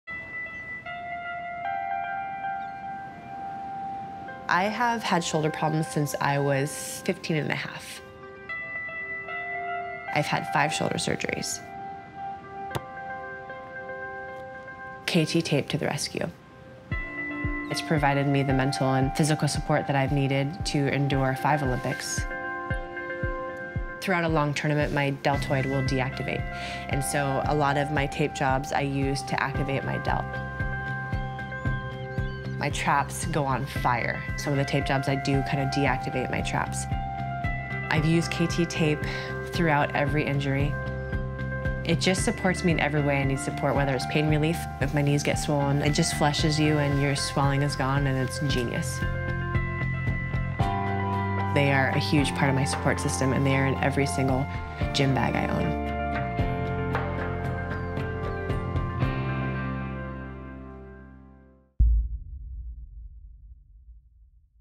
Kerri Walsh Jennings | To The Rescue
Kerri Walsh Jennings has had five shoulder surgeries throughout her career, but she hasn’t let that slow her down. KT Tape is essential to her career and support system.